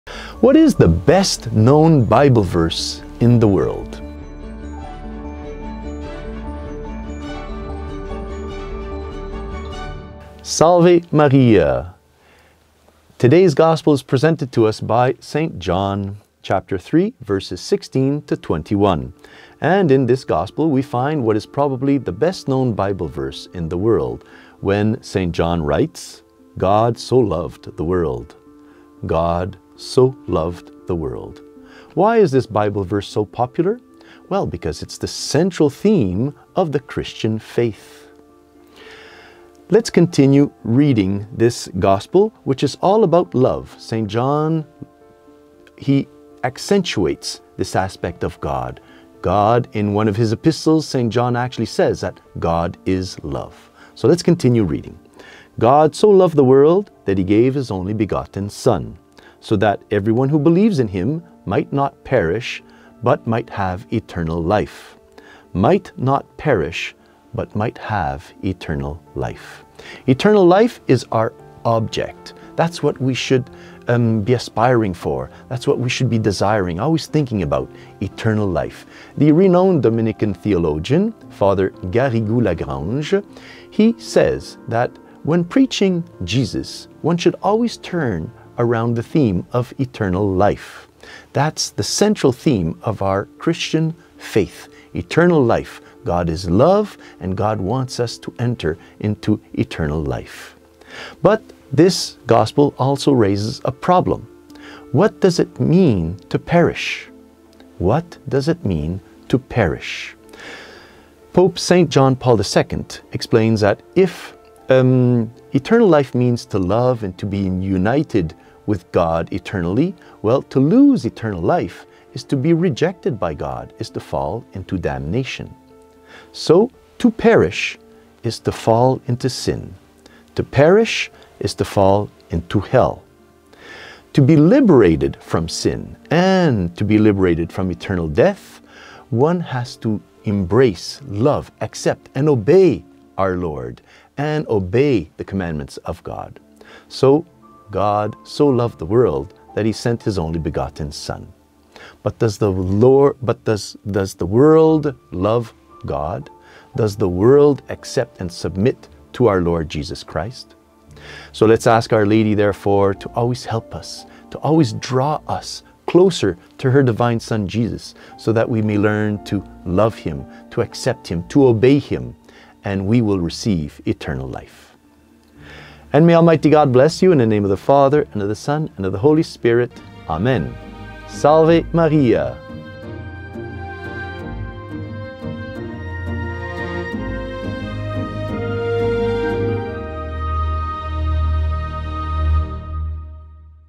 Daily Gospel Meditation | Wednesday of the Second Week of Easter | April 27th, 2022
Eternal Life or Eternal Death?

Spiritual Reading

Please share this message with your family, friends and fellow contacts.